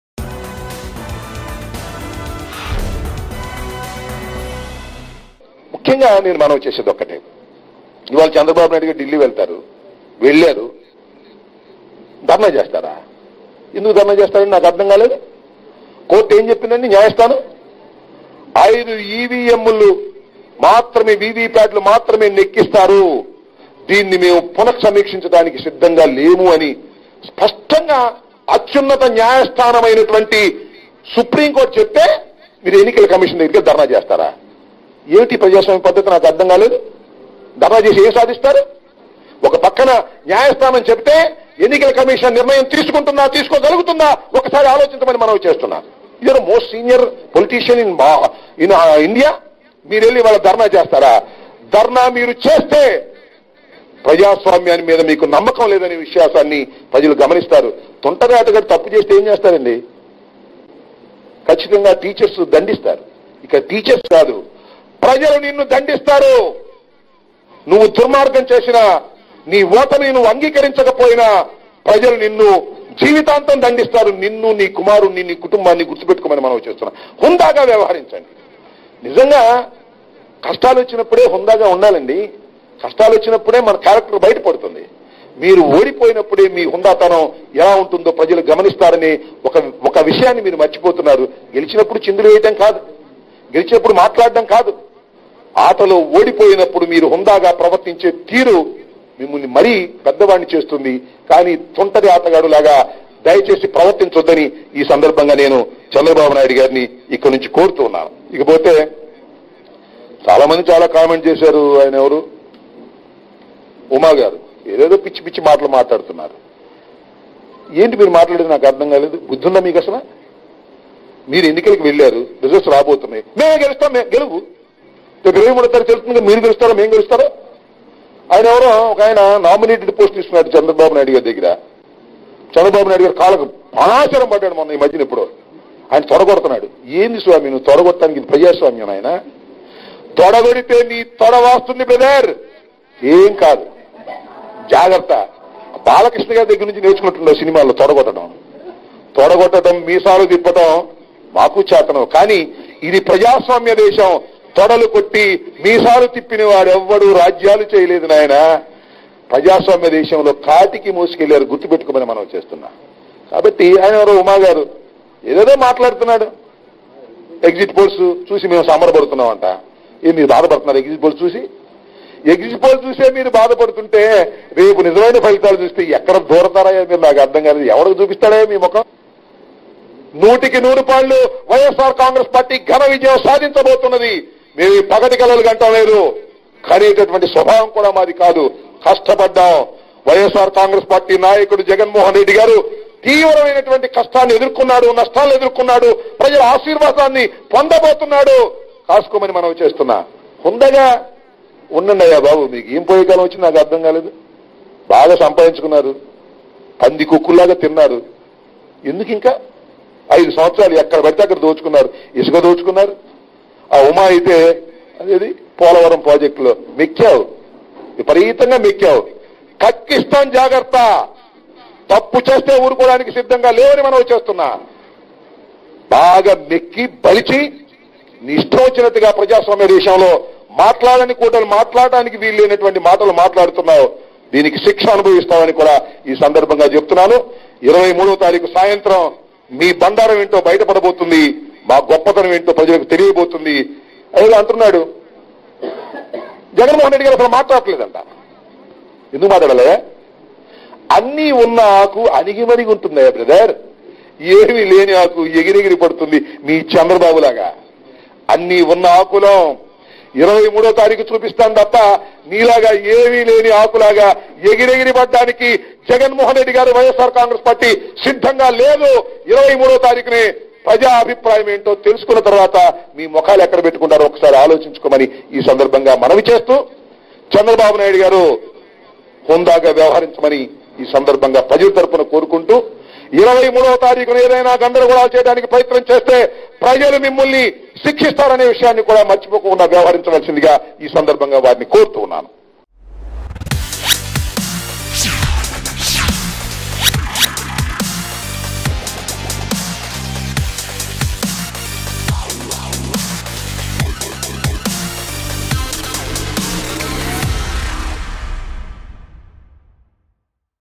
Ambati Rambabu Fires On Chandrababu Naidu And Lokesh Over His Political Strategy |Mango News Telugu
Ambati Rambabu Fires On Chandrababu Naidu And Lokesh Over His Political Strategy.